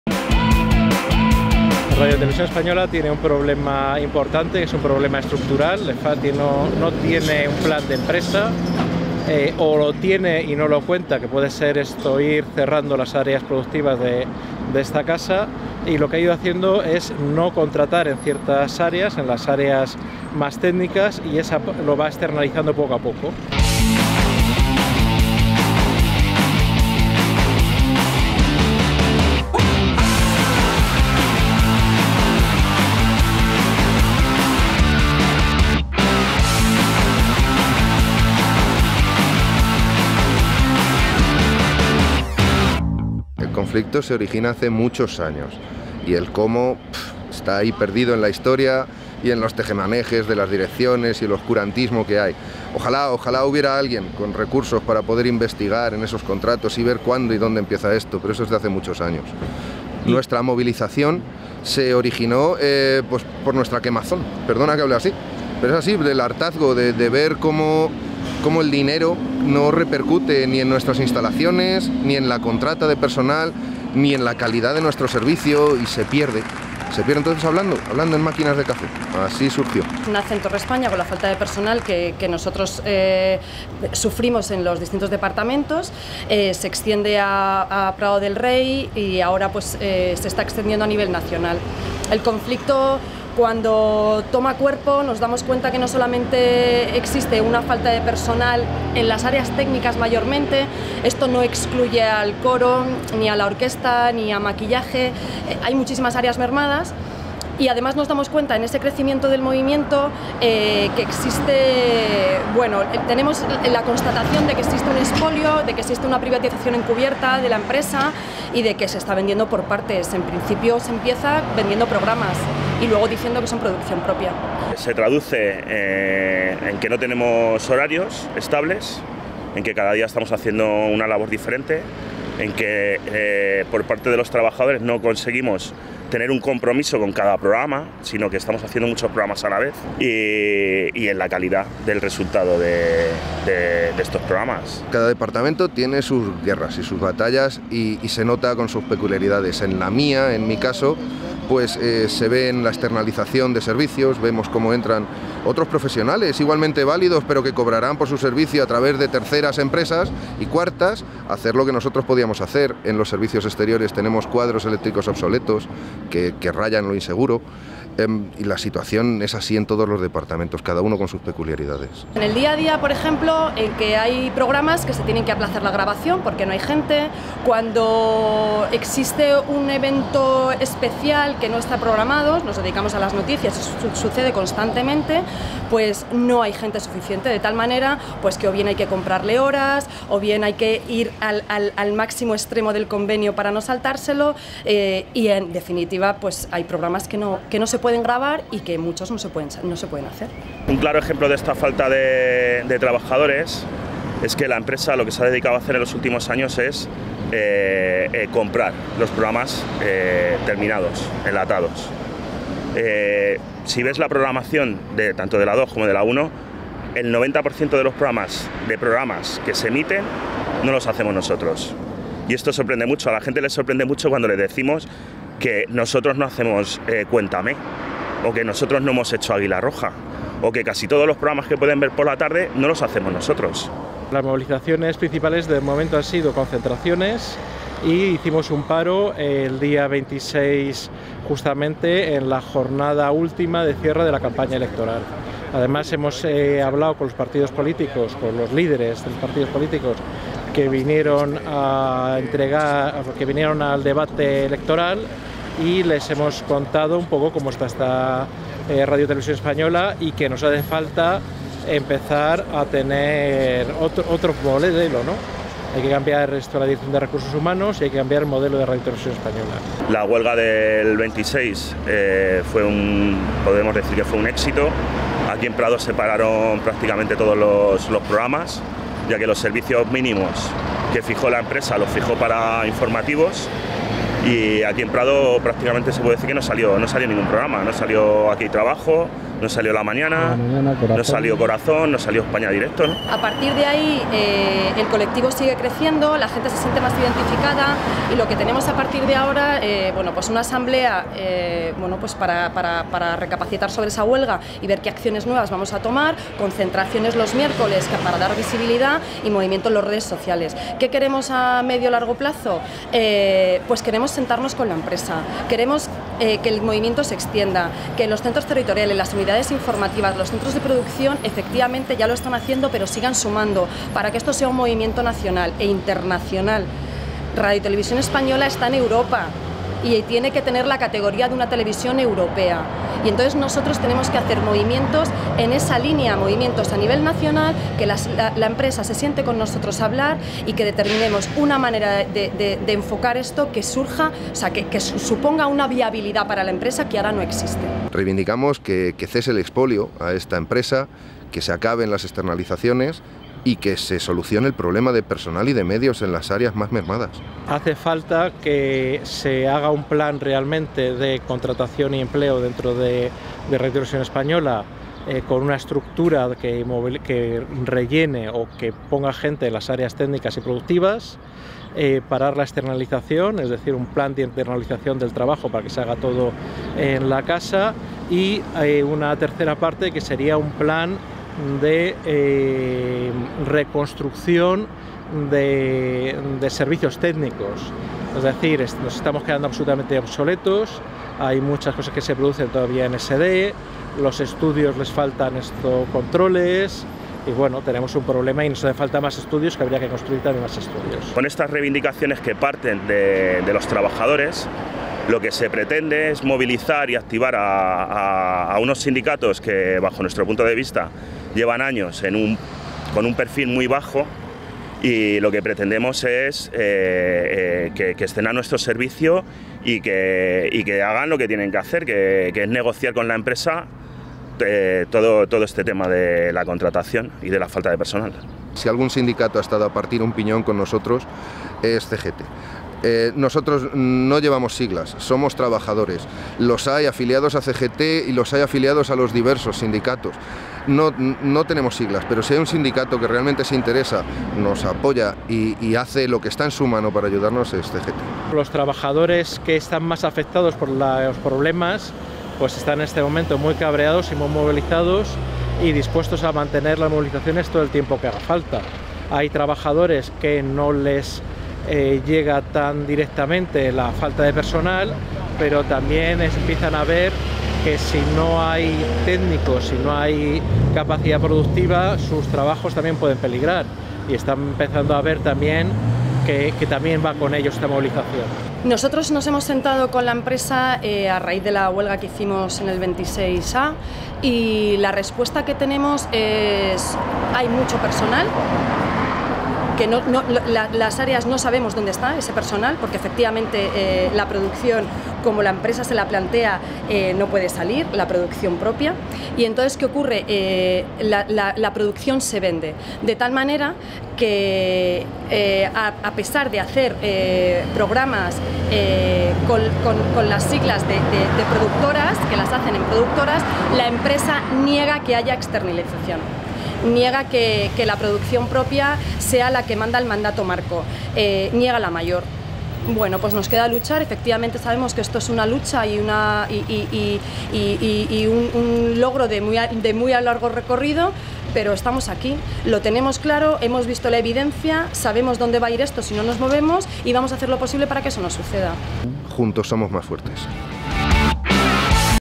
RNtvLucha RTVE
RTVE: arrastra una falta de contratación de la producción. Esta situación ha generado un movimiento interno de protesta y lucha, apoyado por CGT, para exigir más contratación y más inversión técnica.